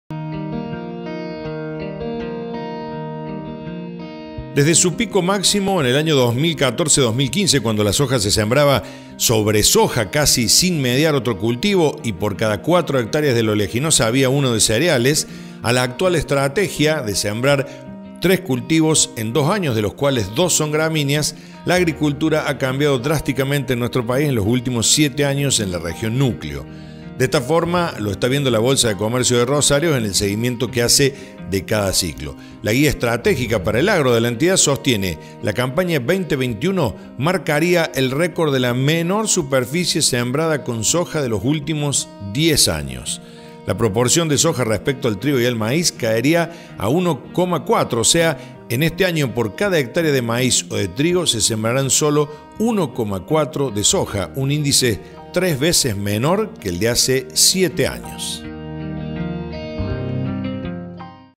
LA SOJA PERDIO SU REINADO
El sistema de cultivos agrícolas ha cambiado radicalmente la proporción de cereales y oleaginosas en la decisión de los productores. Aquí todos los datos.